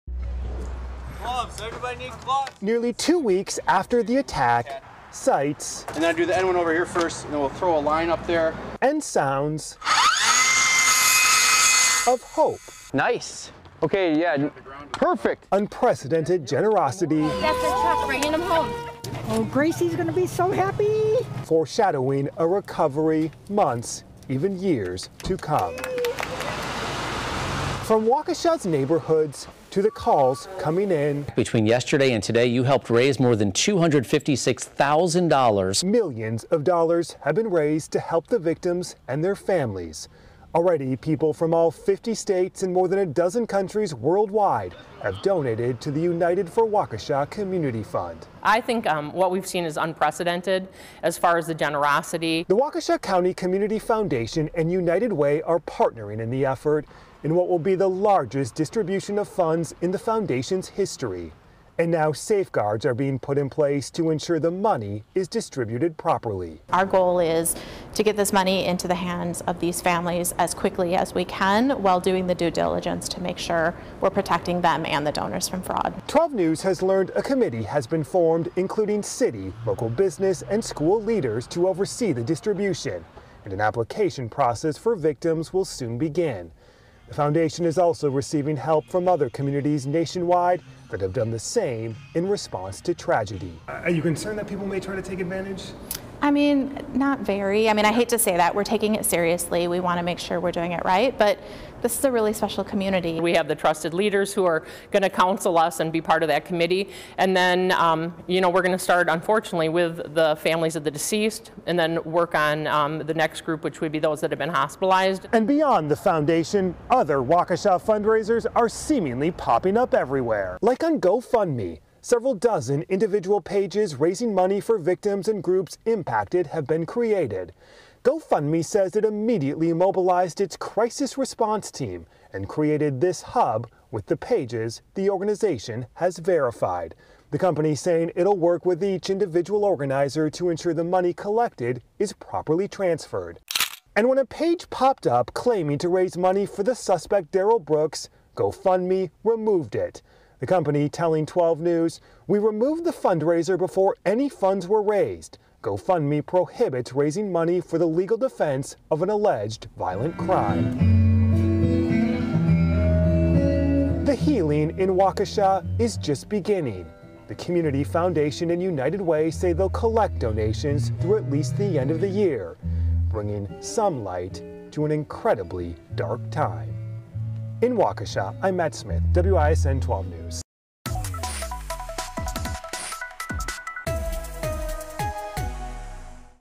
Where are your donations going?
The major undertaking to ensure the money for the Waukesha parade attack victims is distributed properly.